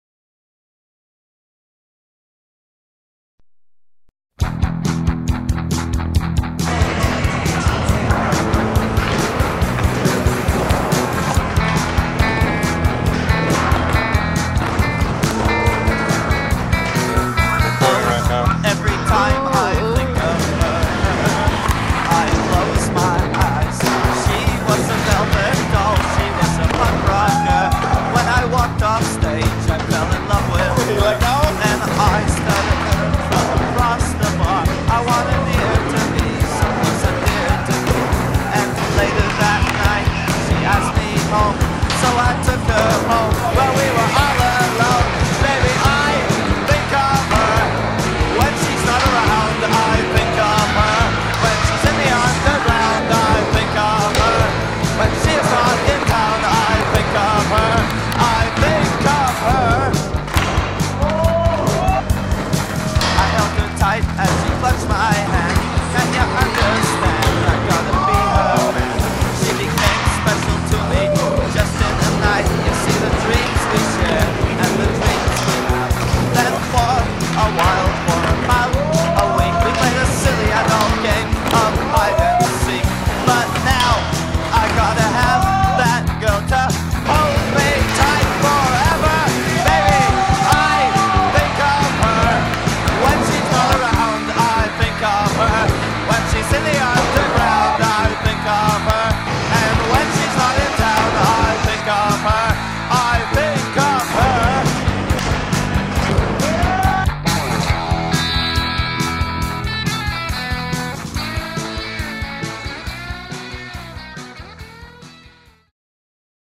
Skateboarding - R.I.P Shred Central - Toronto Ontario Canada
Here is some old footage. Good times have been had at Shred and it was a decade of awesomeness!!!

All shot and edited by the one and only Mr. Essner a.k.a The Eclipse who is a fishing Guru, check him out